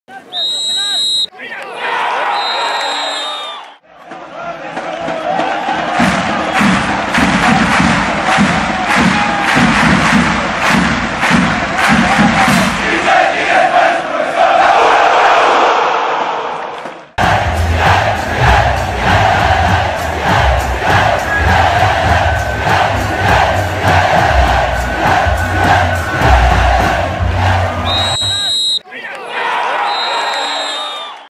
Porras de León y Tigres
Libres y Lokos y Los de Arriba también jugarán la final.